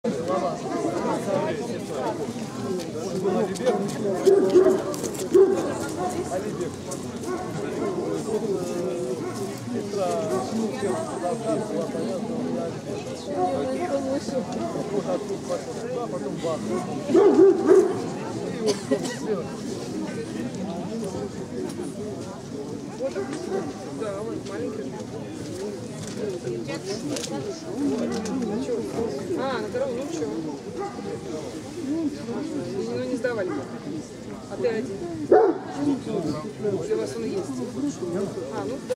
Монопородная выставка САО в г. Александров (13.10.12)
ТРАЙПЛ ТРАСТ ЛАЗЗАТ (о. Егсан из А.С. - м. Ийя Айша Кине от Анверон), д.р. 29.12.10, зав. Глазунова, вл. Варзалова